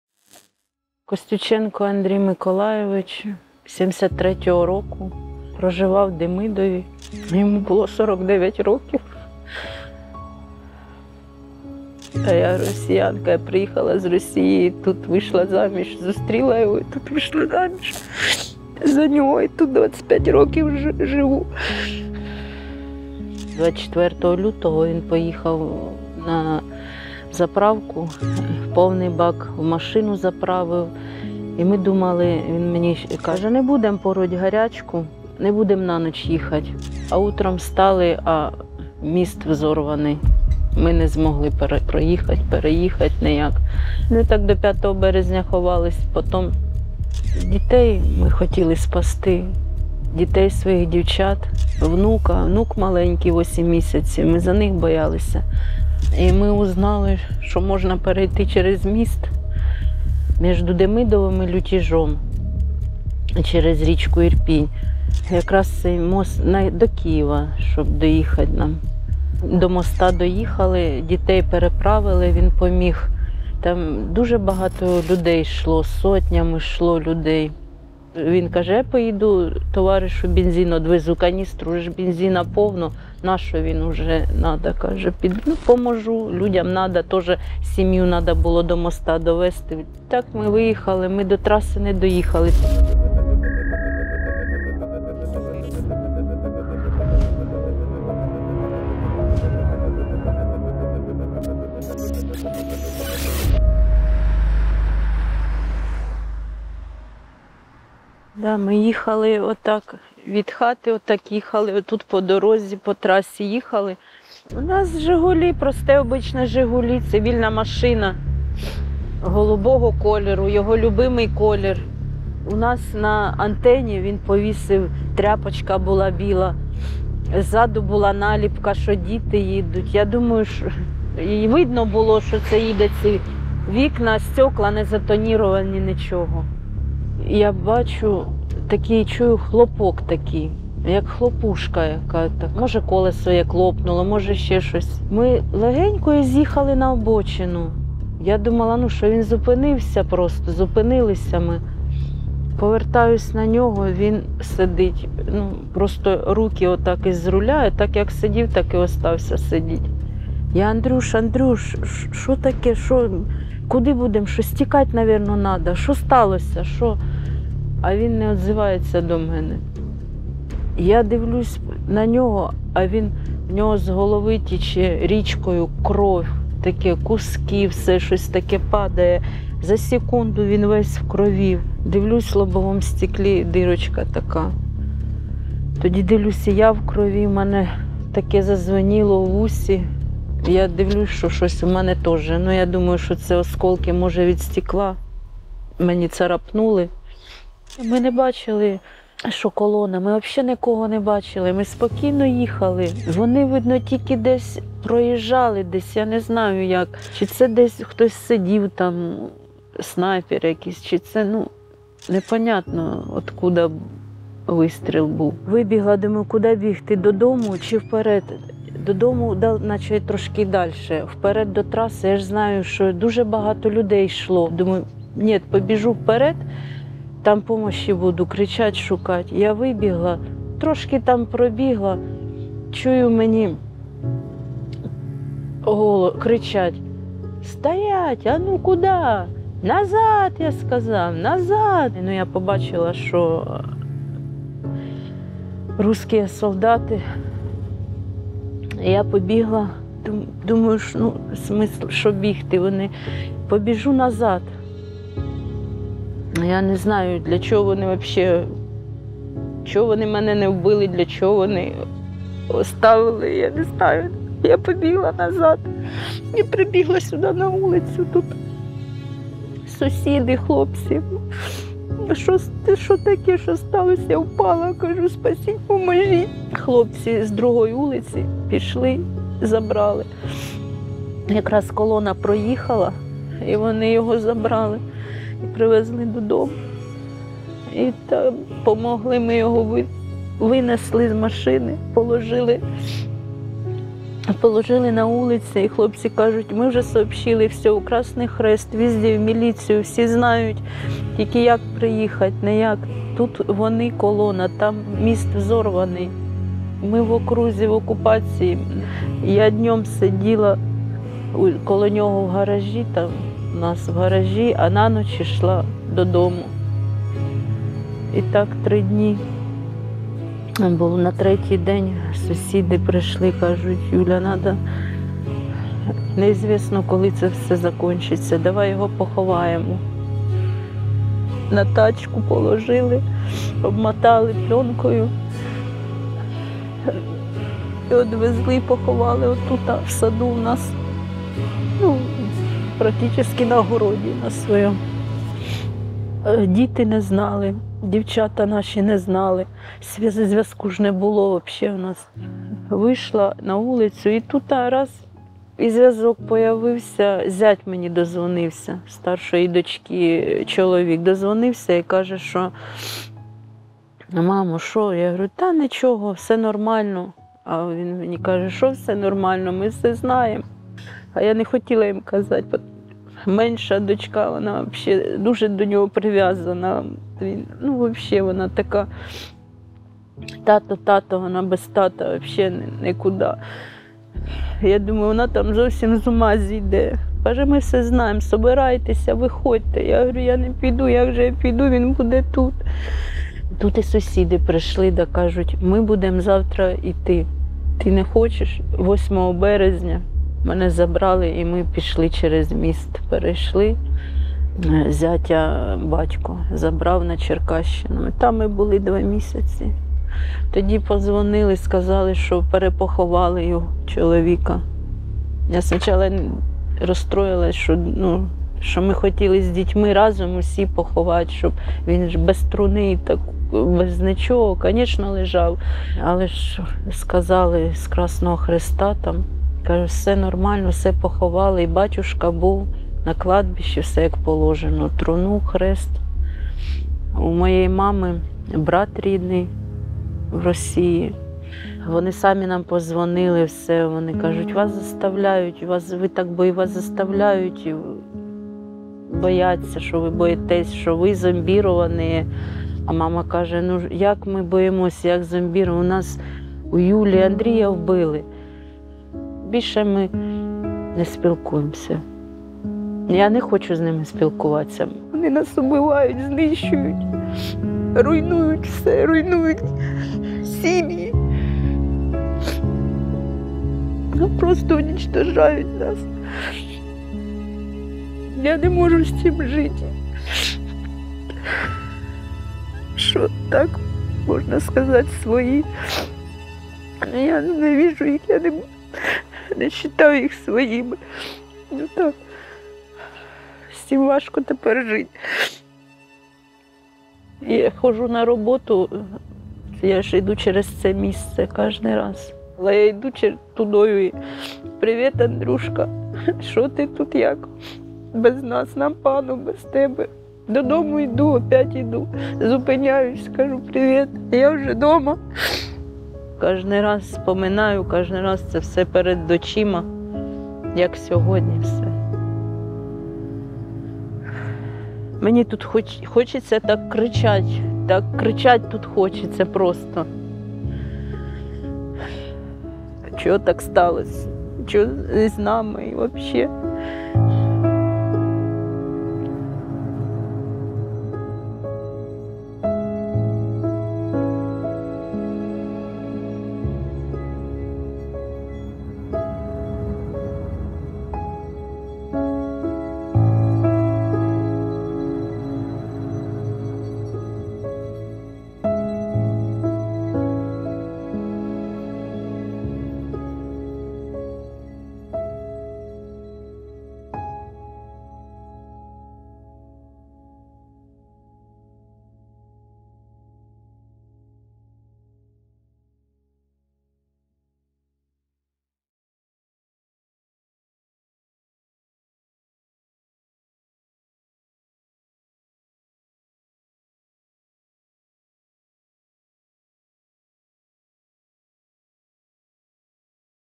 Історія вдови, яка поховала чоловіка в окупації
Сім’я Костюченків жили в селі Демидів під час окупації. 24 лютого вони заправили авто й вирішили рятувати дітей та восьмимісячного внука. До березня переховувались і на початку місяця переправили дітей через напівзруйнований міст через річку Ірпінь. Коли діти перейшли на інший бік ріки, Андрій, їхній батько, вирішив завести залишки пального другу, аби той теж перевіз сім’ю. По дорозі в Андрія вистрілили, дружині, яка сиділа поруч з ним в авто, вдалось втекти. «Я досі запитую себе про те, чому я вижила, чому мене росіяни не застрелили», – каже Юлія.

Матеріал створений за підтримки Ради Європи. Погляди, викладені в ньому, є відповідальністю його авторів і можуть не співпадати з офіційною політикою Ради Європи.